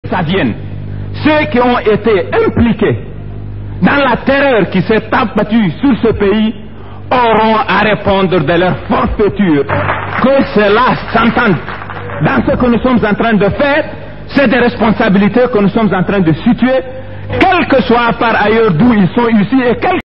Bazoum Mohamed lors de la conférence nationale 1990
ce fervent enseignant en philosophie, s'exprime en rappelant aux uns et aux autres de bien faire leur travail car le tribunal de l'histoire est là , et ce tribunal est incorruptible.